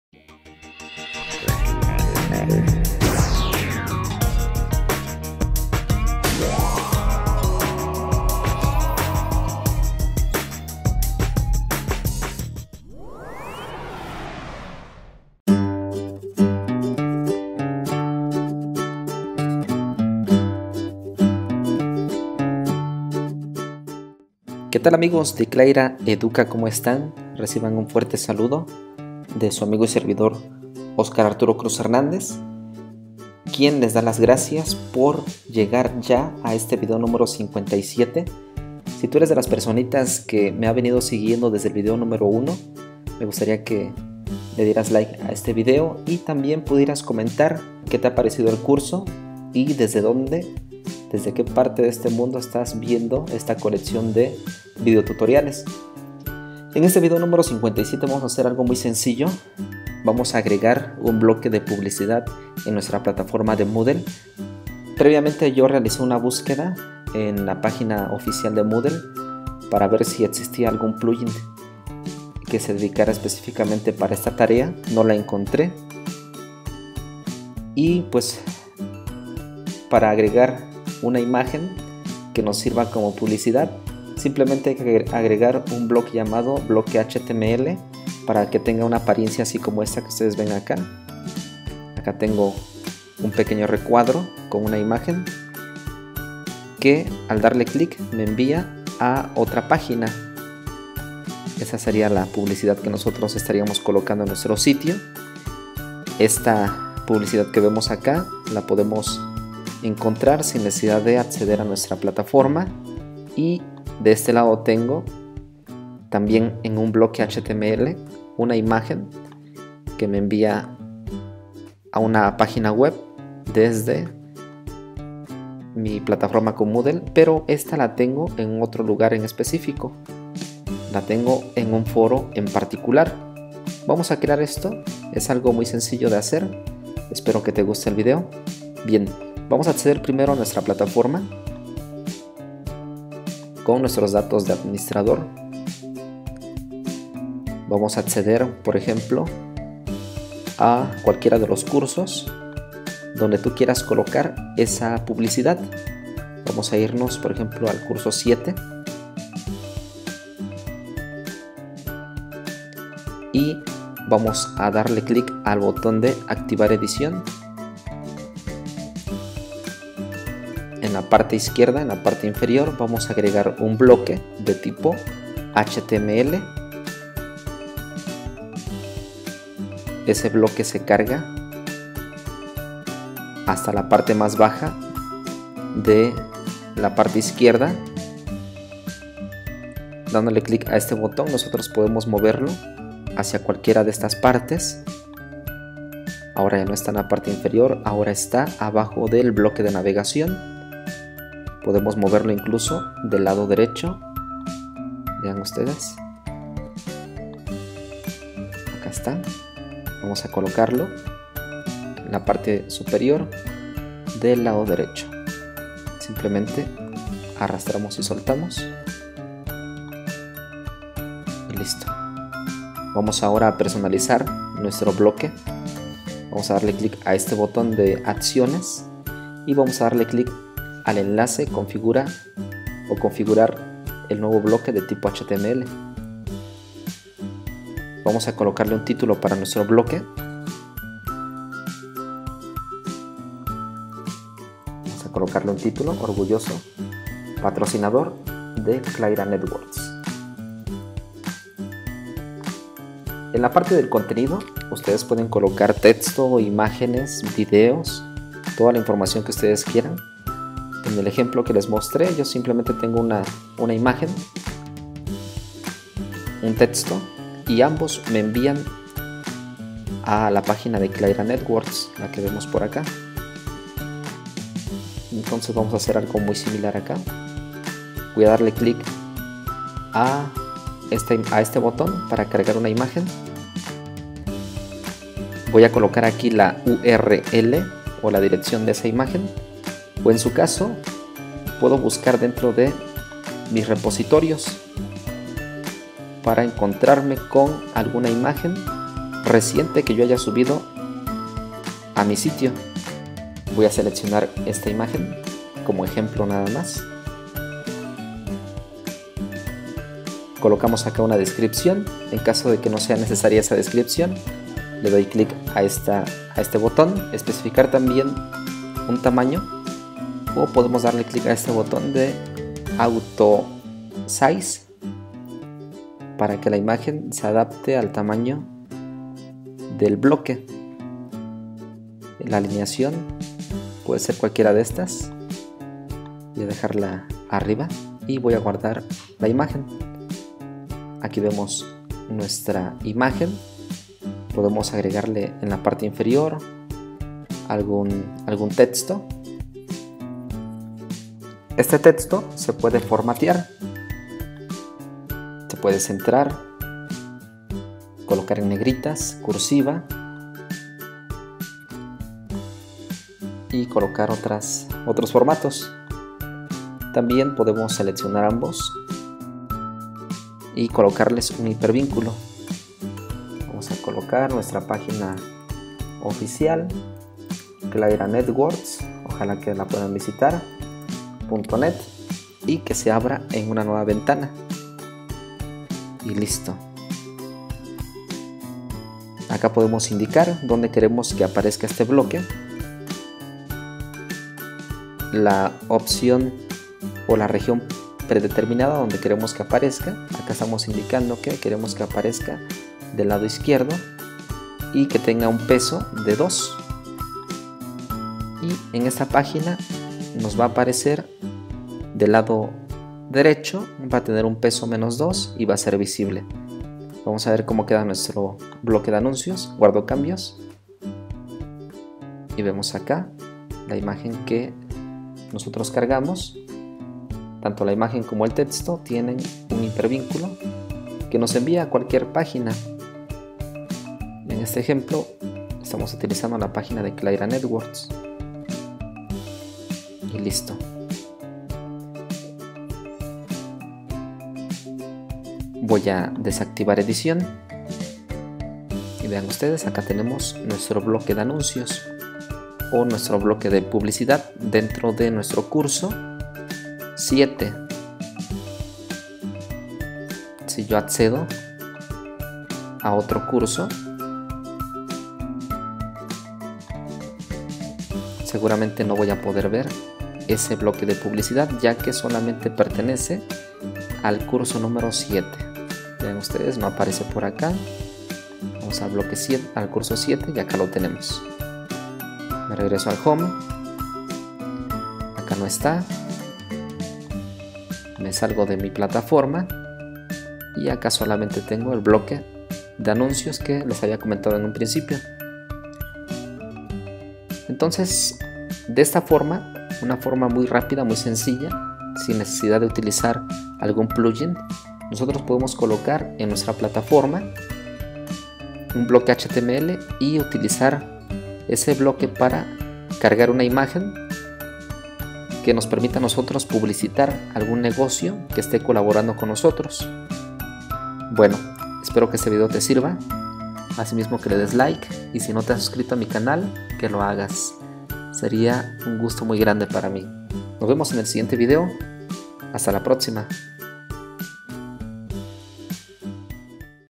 Curso Moodle 3.1 Video 57. Agrega fácilmente un Bloque de Publicidad en Moodle
Tu también puedes ayudar a este proyecto, visita y realiza tu aportación. En los siguientes videos te daré un agradecimiento especial.

Saludos